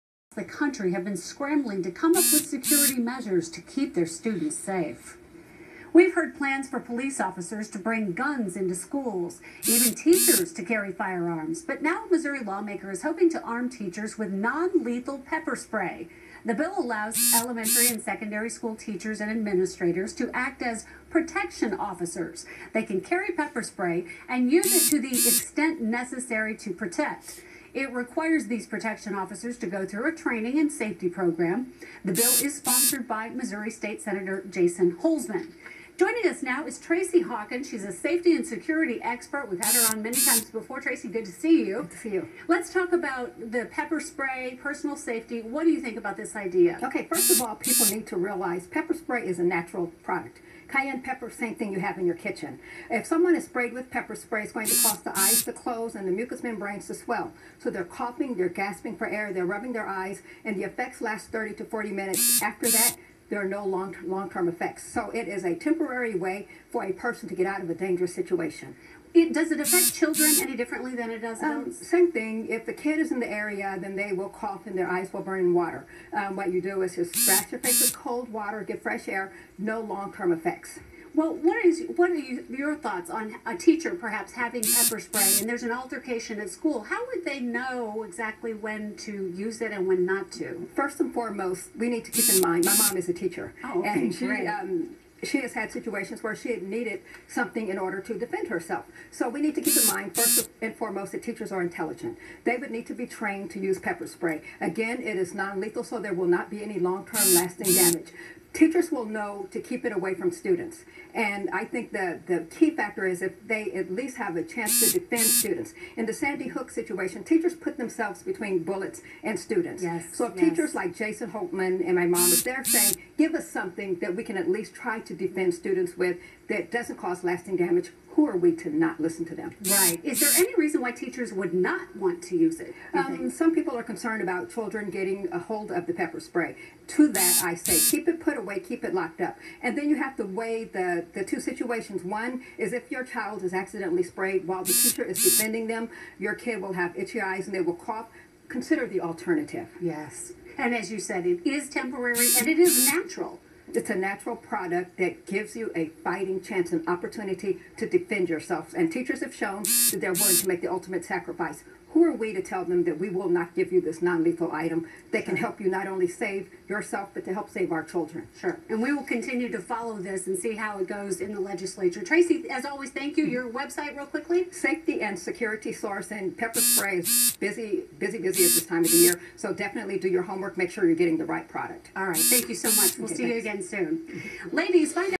Arm Teachers with Pepper Spray
"Tracey, the Safety Lady" advocates in support of Missouri politician's fight to arm teachers with pepper spray.